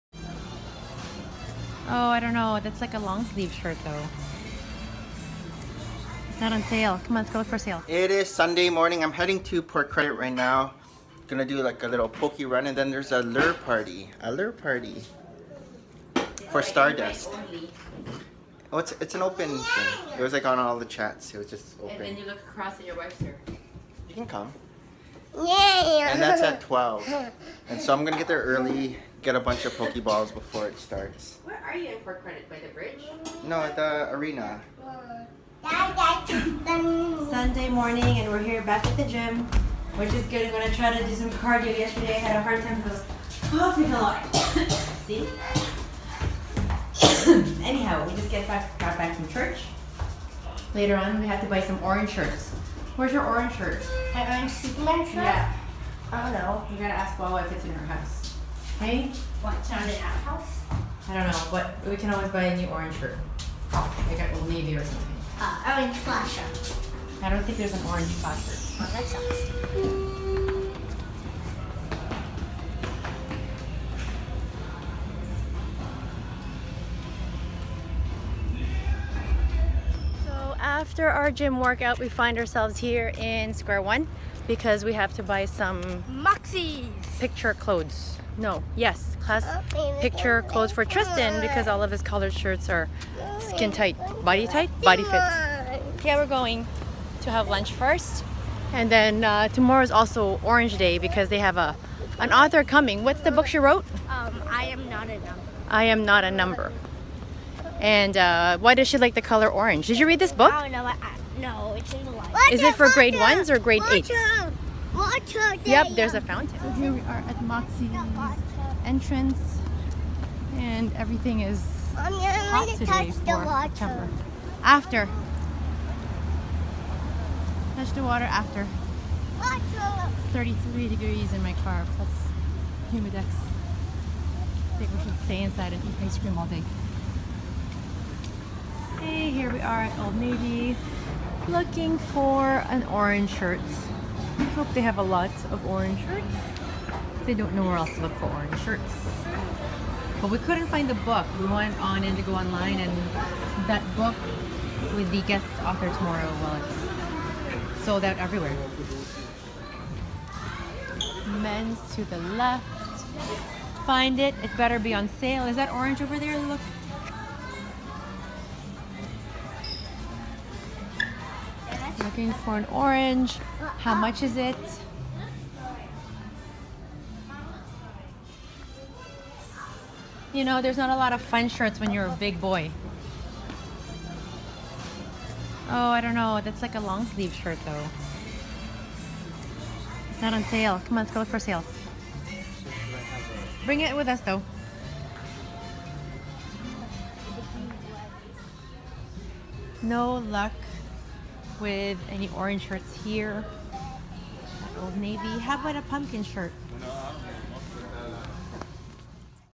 Shopping for Orange Shirts- Daily VLOG #1012 (Sept 24/17)
Tristan runs in the 3K Prediction Run